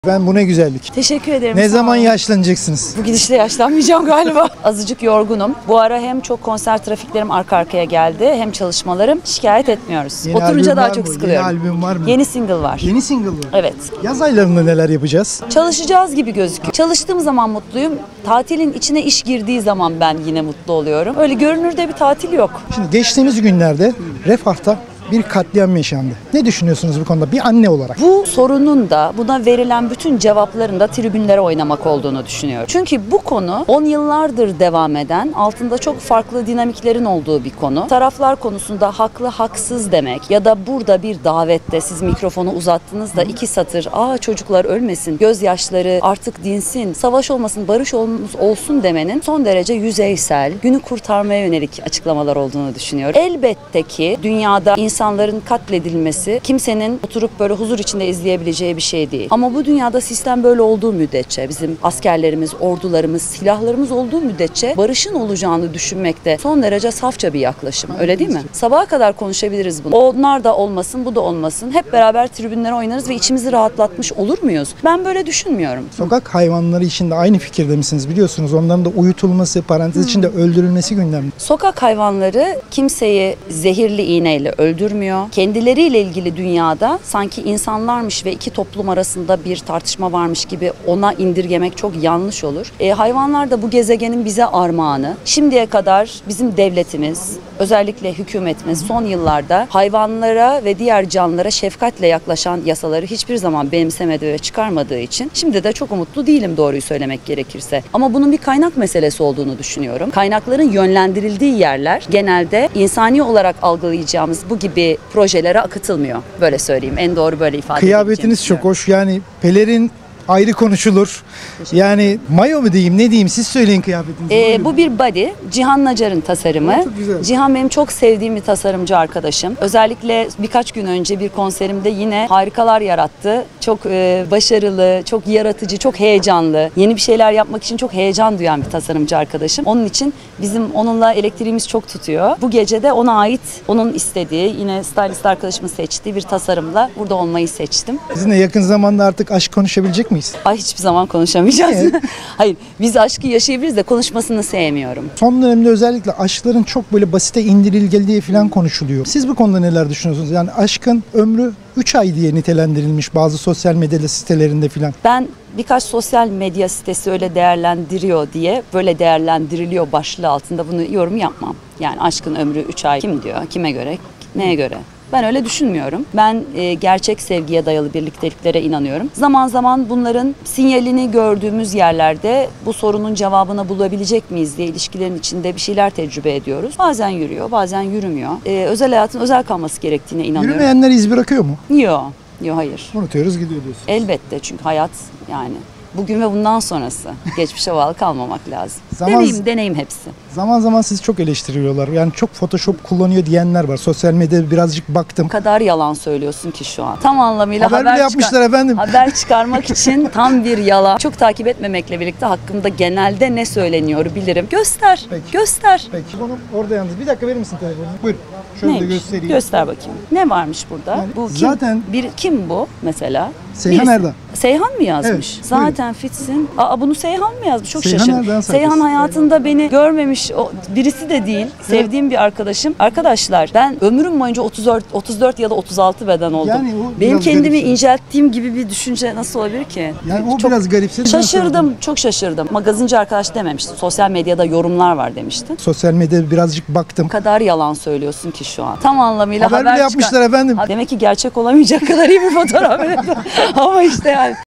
ÖZEL HABER: Defne Samyeli'den Refah'taki katliam hakkında dikkat çeken yorum: Tribünlere oynuyorlar!
Bir markanın özel davetine katılan Defne Samyeli, Magazin Burada mikrofonlarına çok konuşulacak açıklamalar yaptı..

Ünlü manken ve şarkıcı, Refah'taki yaşanan katliam hakkında ''Bu sorunun da, bu konuyla ilgili verilen cevaplarında tribünlere oynamak olduğunu düşünüyorum.'' sözleriyle dikkat çekti.